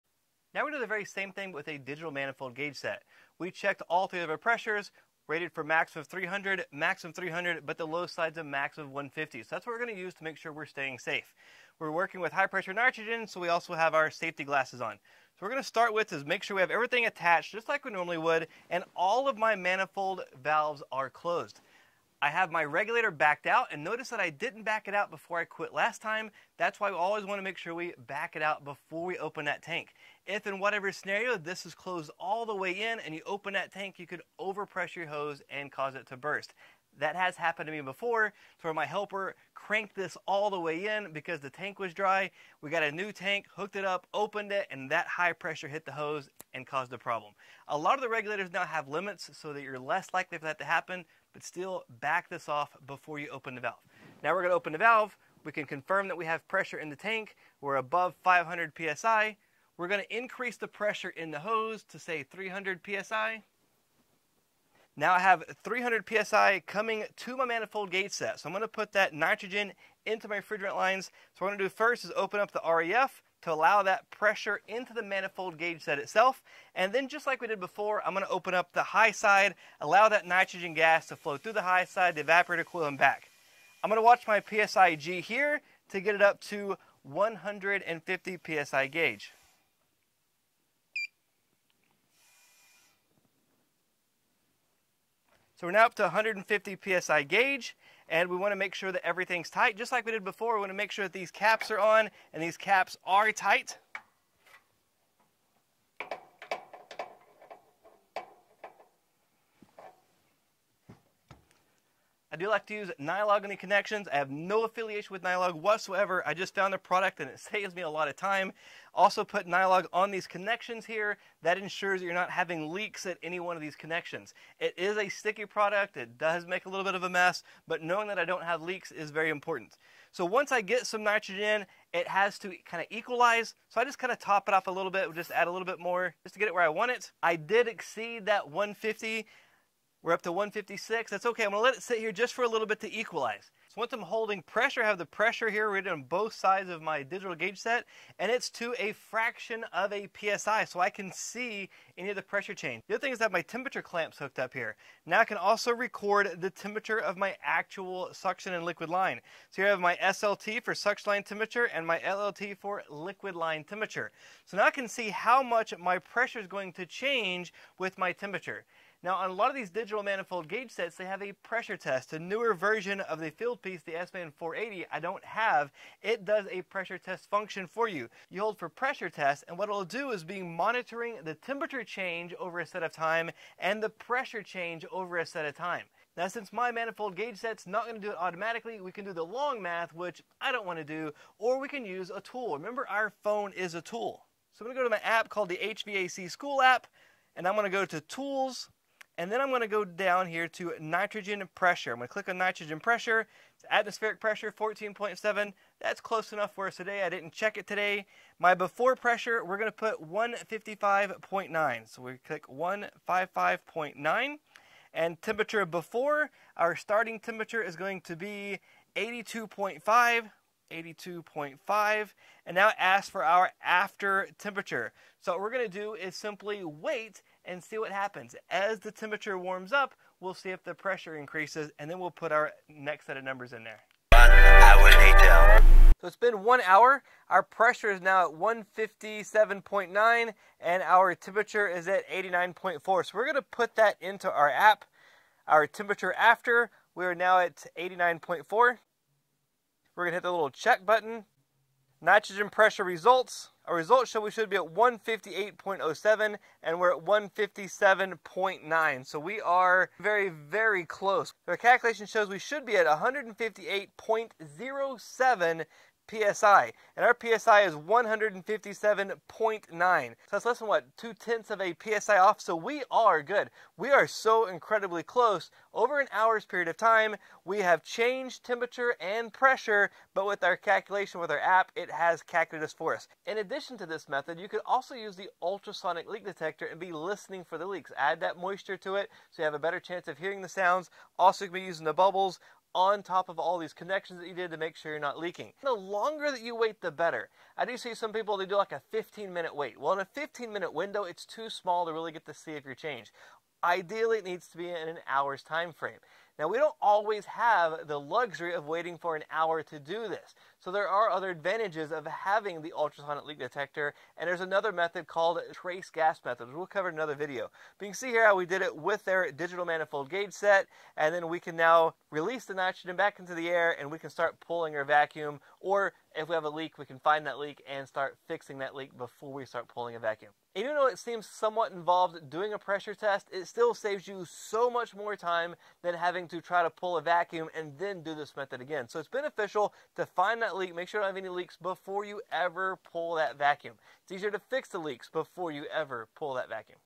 HVAC 154 Nitrogen Test temperature compensation, digital gauges & app
part 2 Continued but now with Digital gauges and temperature compensation app.
With digital gauges we can accurately see the pressure change down to a decimal of a PSI. we can also use the temperature clamps to accurately record the suction line temperature.
Combined with the HVAC school app we can get the accurate pressure with a changing temperature.

Remember nitrogen does follow the gas laws so temperature will affect the pressure. there is a long formula for this but Bryan Orr has simplified this with the HVAC School app.

Here is a better video from my friend Bryan Orr with @HVACS that covers a lot more

Here is an example of @coldfinger459sub0 using the pressure test method of his Sman480 manifold set on an automotive application. Its the newer version of mine.